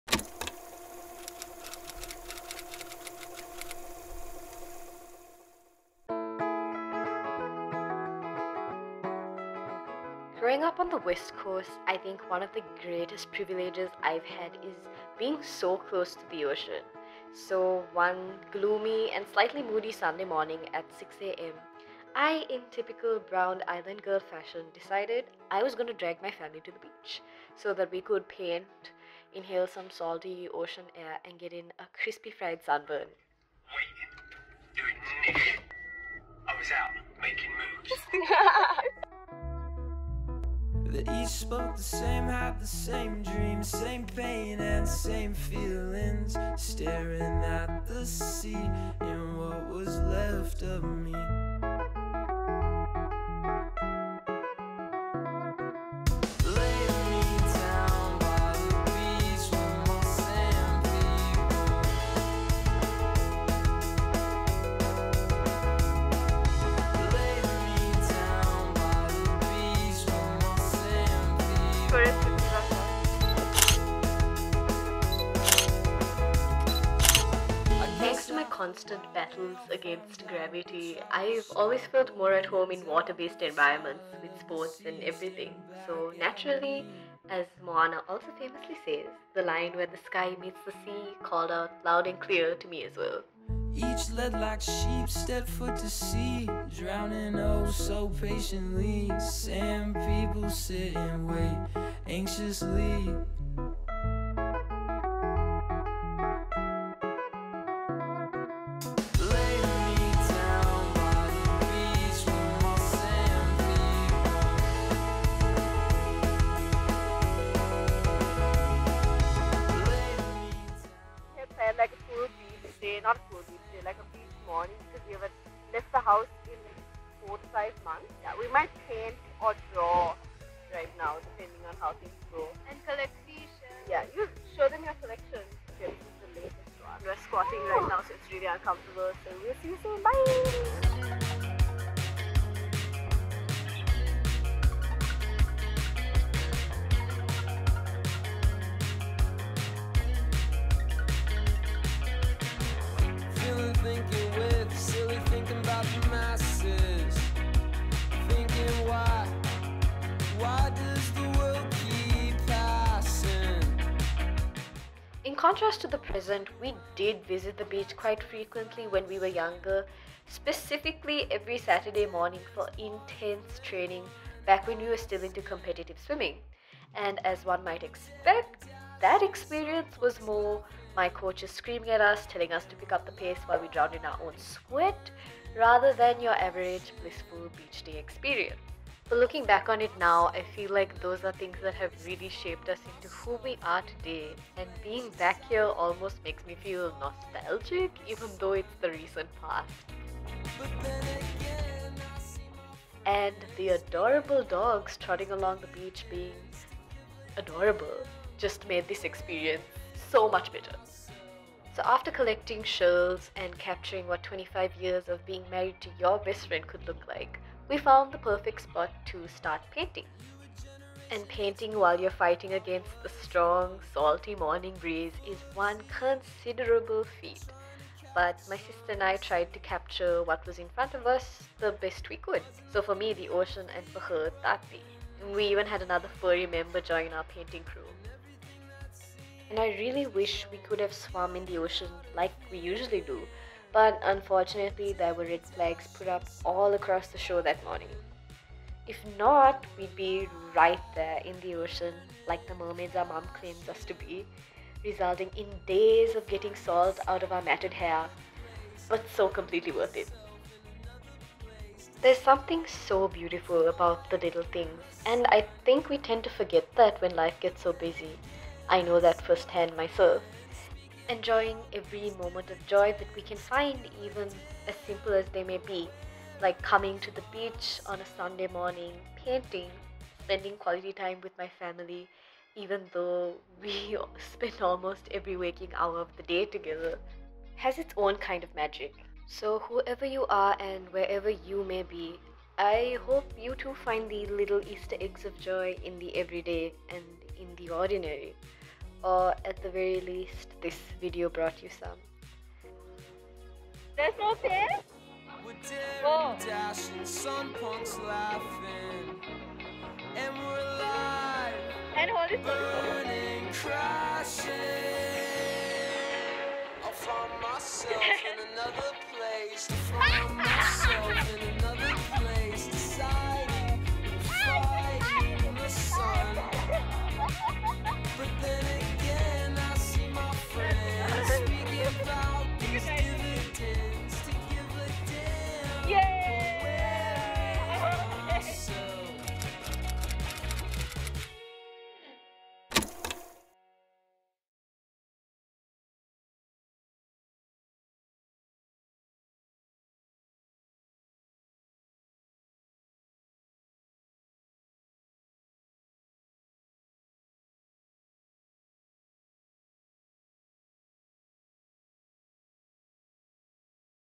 Island mornings in Sri Lanka 🏝 painting at the beach🎨✨ Vlog 003
Hi theree welcome to a completely regular Sunday morning at the beach on Sri Lanka (make sure to watch in 1080p)
Where we try to make the most of our close proximity to the ocean, soo come along as I drag my family out to the ocean and find the joys of the everyday and the ordinary. It’s been a rough few months for a lot of people so I hope this video inspires you to find little Easter eggs of happiness wherever you go✨ and if you made it this far make sure to comment what little things make you happy 🤓 thanks for being here and I’ll see you next week ❤️
Jé

Camera - i phone Xs max